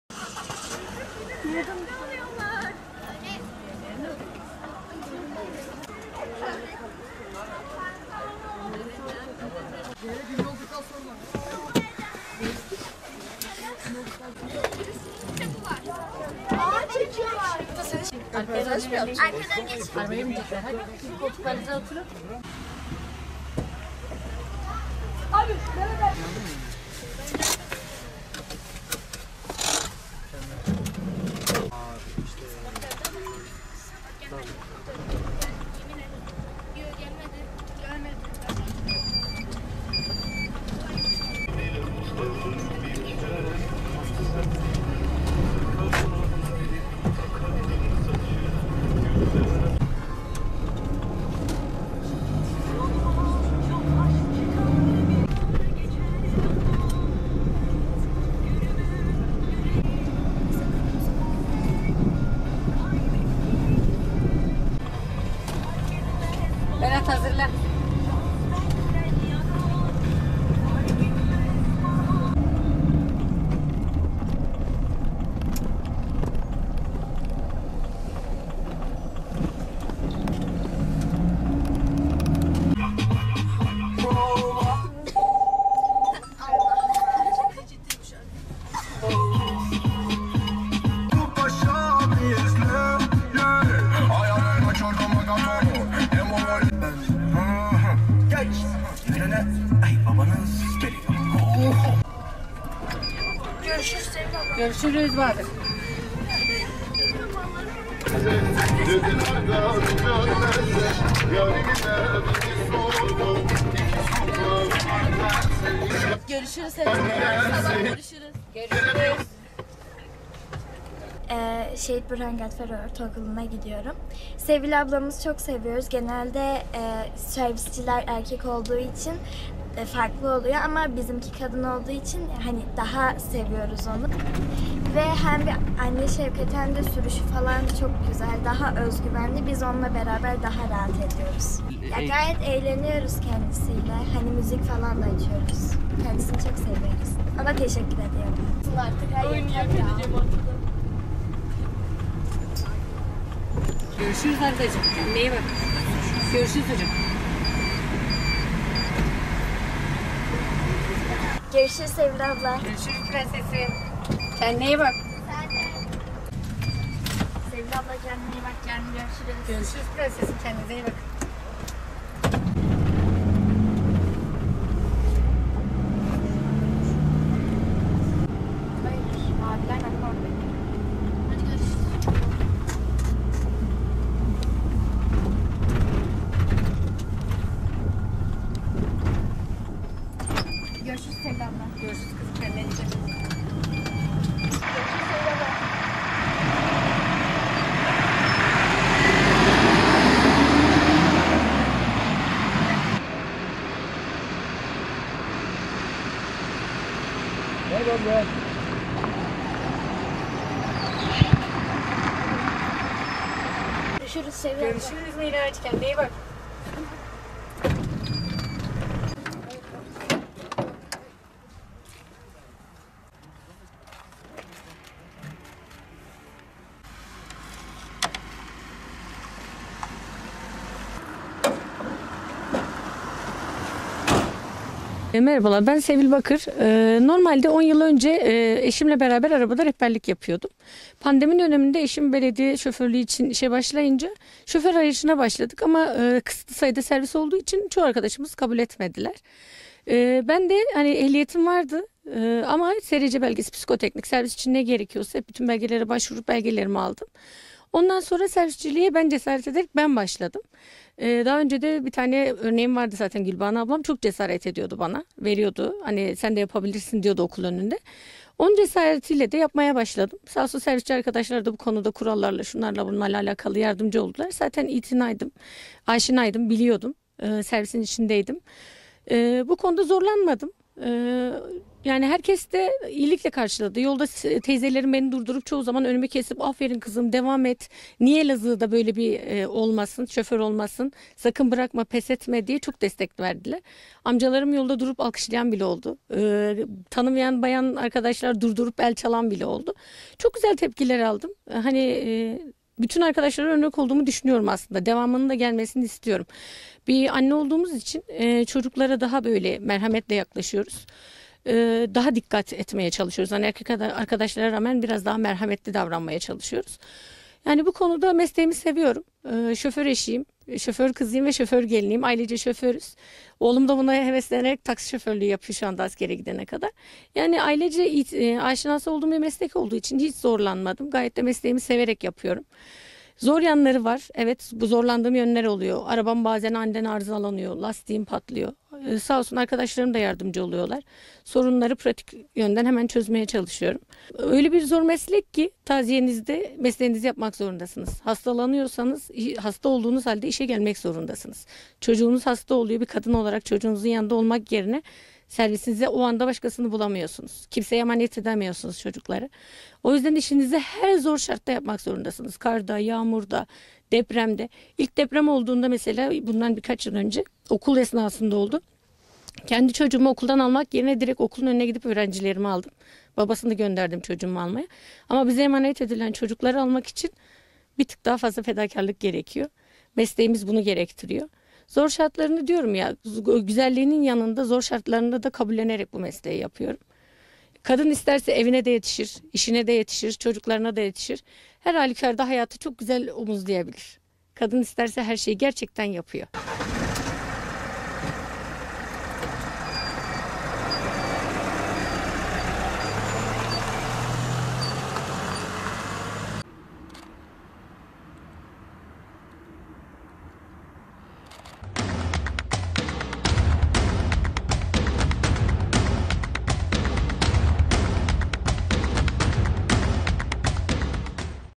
Öğrencileri anne şefkatiyle okullarına ulaştırıyor!
Elazığ’da servis şoförlüğü yapan eşinin başka bir işe girmesinin ardından şoför bulamayan 2 çocuk annesi Sevil Bakır, direksiyon başına geçti. Çocukları bir anne şefkatiyle okullarına ulaştıran Bakır, 4 yıldır görevini başarı ile sürdürüyor.

Elazığ’da yaşayan 2 çocuk annesi Sevil Bakır (41), eşinin servisinde uzun yıllar rehberlik yaptı. Eşinin belediye işe girmesinin ardından Bakır, aracına şoför aradı ancak bulamadı. Bunun üzerine ehliyetini yükselten Bakır, direksiyon başına geçti. Bakır, hafta içi her sabah öğrencileri okula yetiştirmek için mesai harcıyor. Öğrencilerini kendi çocukları gibi gören Bakır, adeta bir anne şefkati ile okullarına ulaştırıyor. Çok az sayıda kadın şoförden birisi olan ve mesleğini severek yapan Bakır, adeta erkeklere taş çıkartıyor. Görenlerin bir yandan hayret diğer yandan da taktir ettiğini dile getiren Bakır, “Anne olduğum için çocuklara daha merhamet yaklaşıyor, daha çok dikkat ediyorum’ dedi. Depremde kendi çocuğunu okuldan almak yerine öğrencilerini aldığını aktaran Bakır, kadının isterse her şeyi yapabileceğini ifade etti.
10 yıl önce eşinin servisi arasında rehberlik yaptığını belirten Sevil Bakır, “Pandemi döneminde eşim belediyede şoför olarak işe başlayınca çalışan arayışına başladık. Ama kısıtlı sayıda servis olduğu için çoğu şoför arkadaşımız kabul etmedi. Benim de ehliyetim vardı. SRC, piskoteknik ne gerekiyorsa başvurup tüm belgeleri aldım. Cesaret ederek servisçiliğe başladım. Daha öncede bir örnek vardı ve bana sende yapabilirsin diye cesaret veriyordu. O cesaretle birlikte yapmaya başladım. Diğer servisçi arkadaşlarda bana yardımcı oldu. Zaten eşimden dolayı biliyordum. Bu konuda zorlanmadım. Herkes güzel karşılıyor. Yolda teyzeler durduruyor ‘aferin kızım devam et, sakın bırakma ve pes etme’ diyor. Yolda durup alkışlayan amcalar bile oldu” diye konuştu.
“Şoför eşi, kızı ve geliniyim. Ailecek şoförüz”
Çok güzel geri dönüşler aldığını aktaran Bakır, “Tüm arkadaşlara örnek olduğumu düşünüyorum. Anne olduğumuz için çocuklara daha merhamet yaklaşıyoruz. Daha çok dikkat ediyoruz. Mesleğimi seviyorum. Şoför eşi, kızı ve geliniyim. Ailecek şoförüz. Oğlumda heveslenerek taksi şoförlüğü yapıyor. Ailecek aşınası olduğum bir meslek olduğu için hiç zorlanmadım. Mesleğimi severek yapıyorum. Zor yanları tabi ki var. Aracım aniden arızalanıyor, lastik patlıyor. Sağ olsun arkadaşlarımda yardımcı oluyor” şeklinde konuştu.
“Kadın isterse her şeyi yapar”
Sorunları pratik olarak hemen çözmeye çalıştığını dile getiren Bakır, “Taziyeniz olsa bile mesleğinizi yapmak zorundasınız. Hasta olsanız daha gelmek zorundasınız. Çocuğunuz hasta oluyor, onun yanında olmak yerine servise kimseyi bulamıyor ve çocukları kimseye emanet edemiyorsunuz. O yüzden işinizi her zor şartta yapmak zorundasınız. Deprem olduğunda kendi çocuğumu okuldan almak yerine okulun önüne gidip öğrencilerimi aldım. Bize emanet edilen çocuklar için daha fazla fedakarlık gerekiyor. Mesleğimiz bunu gerektiriyor. Güzelliklerinin yanında zorluklarını da katlanarak bu mesleği yapıyorum. Kadın isterse evine de, işine de, çocuklarına da yetişir. Dışarıda hayatı çok güzel omuzlayabilir. Kadın isterse her şeyi yapar” dedi.
“Bize anne şefkati ile yaklaşıyor ve sürüşü daha güzel”
7'nci sınıf öğrencisi Özge Türk ise “Sevil ablamızı çok seviyoruz. Genelde servisçiler erkek oluyor ama bizimki kadın. Ondan dolayı daha çok seviyoruz. Anne şefkati ile yaklaşıyor ve sürüşü daha güzel. Daha özgüvenli ve biz onunla daha çok rahat ediyoruz. Kendisi ile eğleniyoruz. Araçta bize müzik açıyor. Kendisini çok seviyoruz” ifadelerini kullandı.